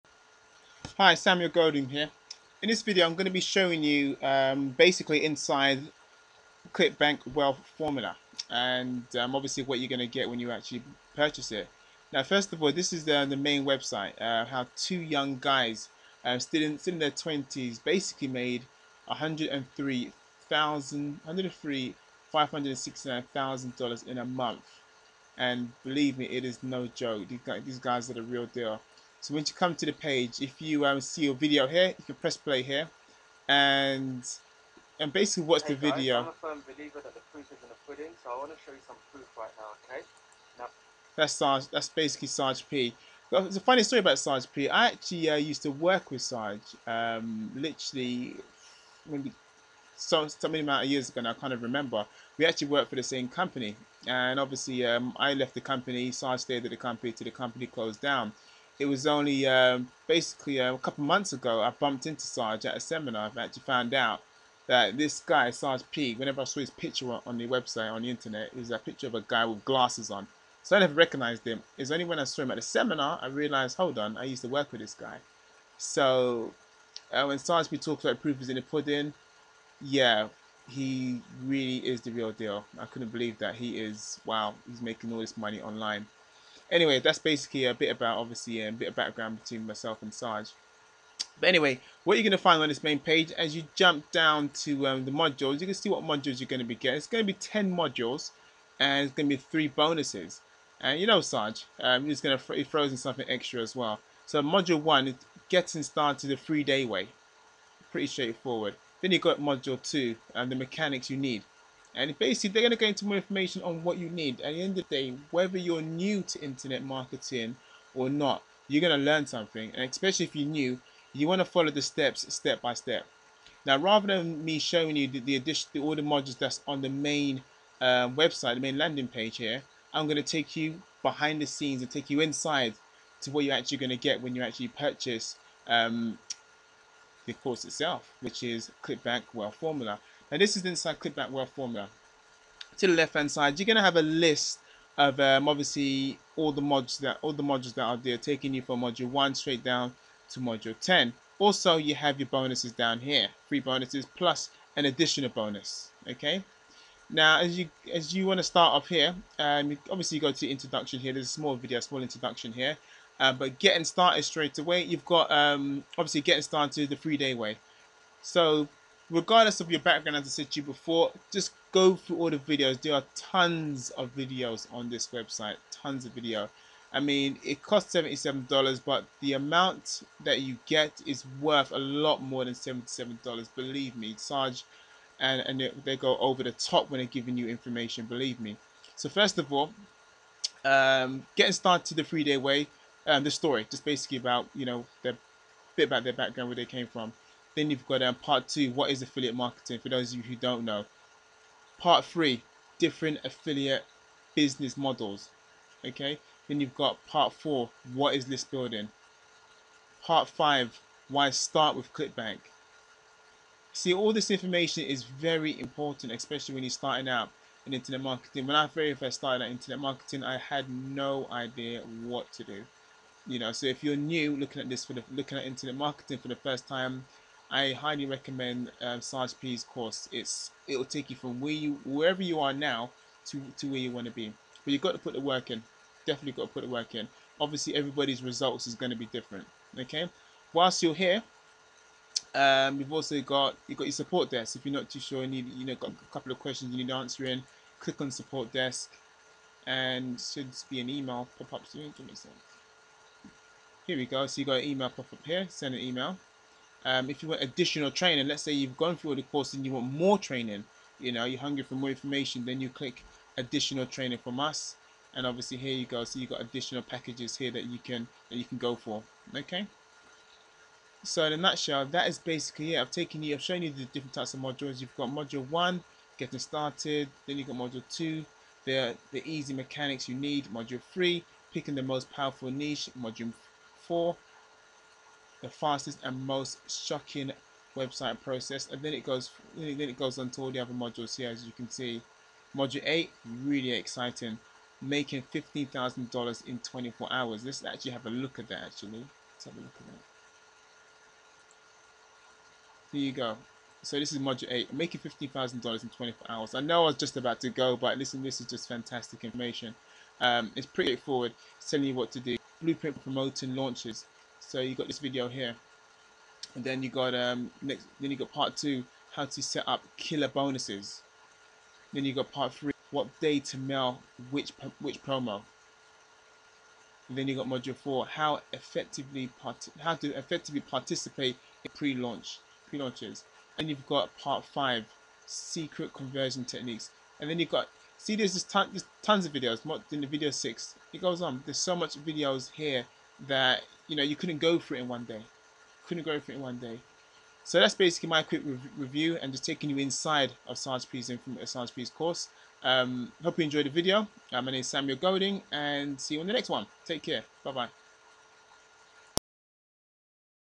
Make Money ClickBank Wealth Formula Review
This short video shows you inside out want you will get from Saj P and Anik Singal's CB Wealth Formula Course.